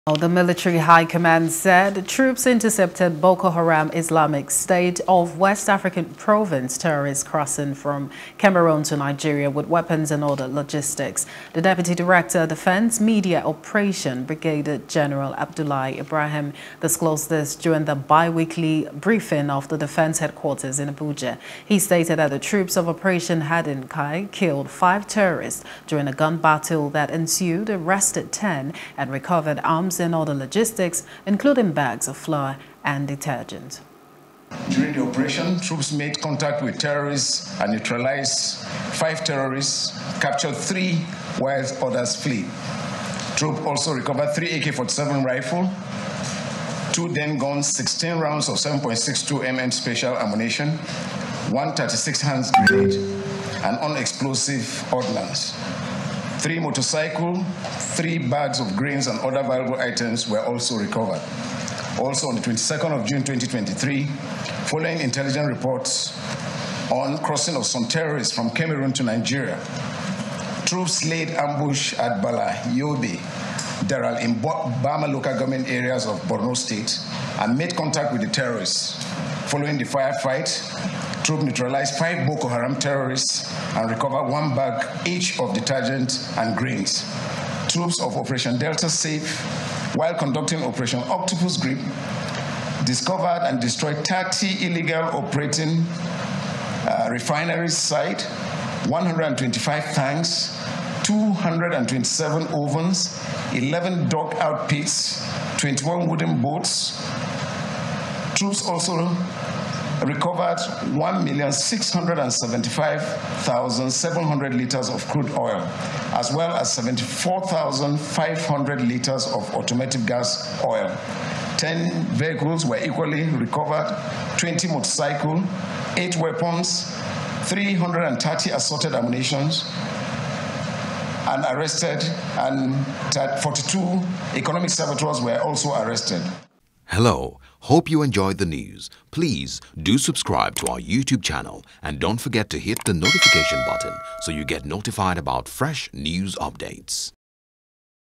Insecurity: Troops Intercept Terrorists, Kill Five
The military high command said troops intercepted Boko Haram/Islamic State of West African Province terrorists crossing from Cameroon to Nigeria with weapons and other logistics.